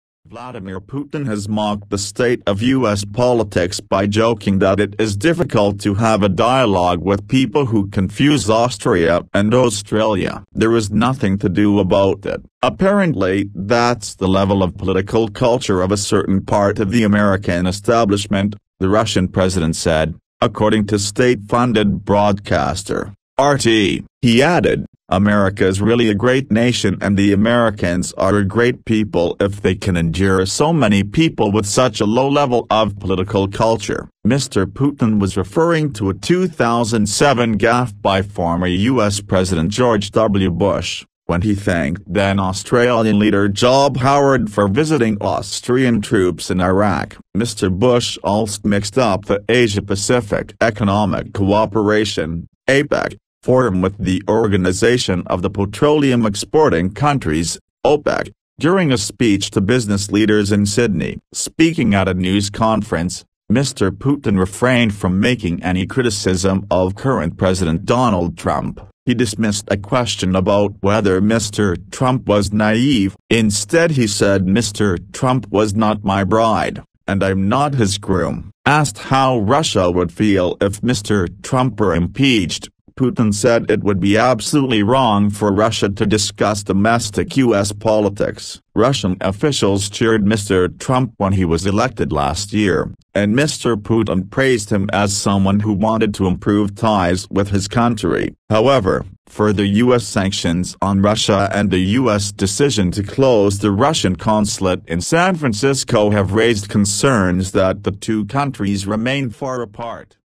Putin on US administration: 'It’s difficult to talk with people who confuse Austria and Australia'.
'Americans are a great people if they can endure so many people with such a low level of political culture,' Russian president says.